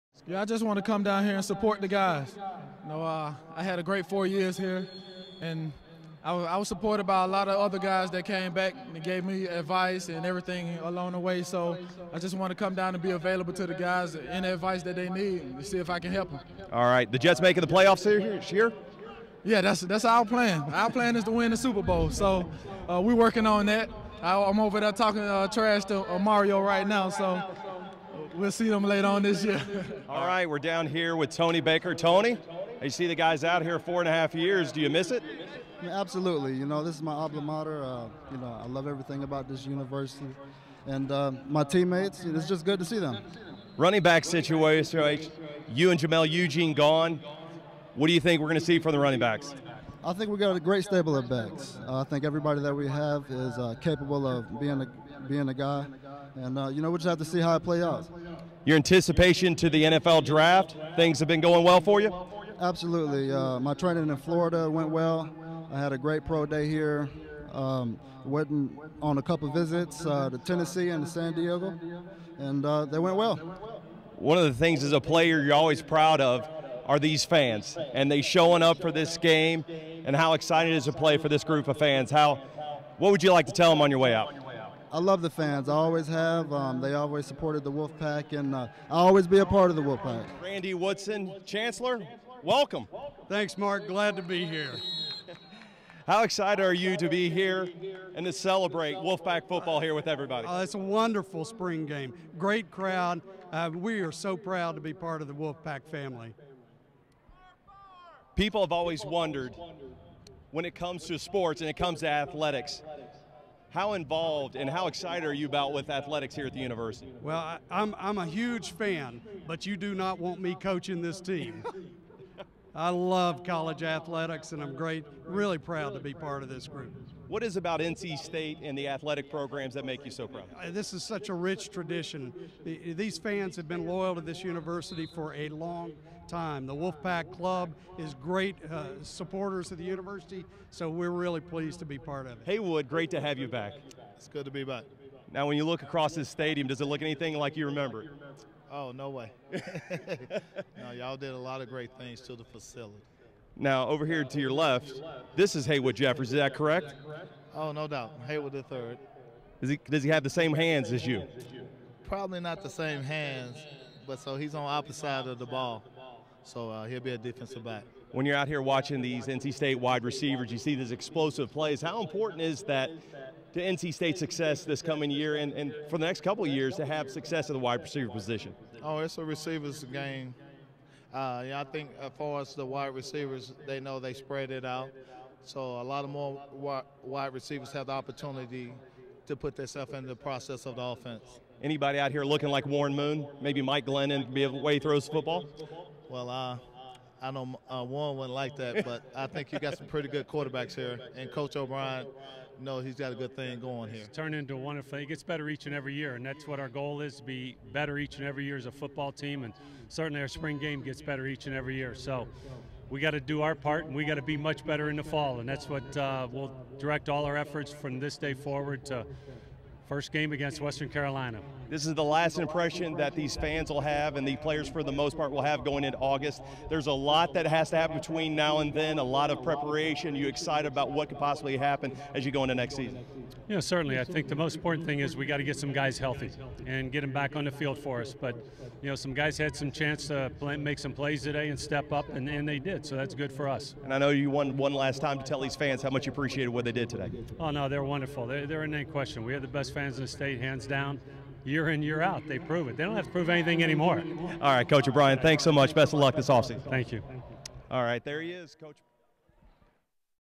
Interviews: 2010 NC State Kay Yow Spring Football Game
A collection of on-field interviews from the 2010 Kay Yow Spring Football Game, held April 17 at Carter-Finley Stadium.

Mark Thomas chats with NC State chancellor Randy Woodson, head coach Tom O'Brien, as well as former Wolfpack stars Haywood Jeffires, Jerricho Cotchery and Toney Baker.

Produced by WolfpackTV.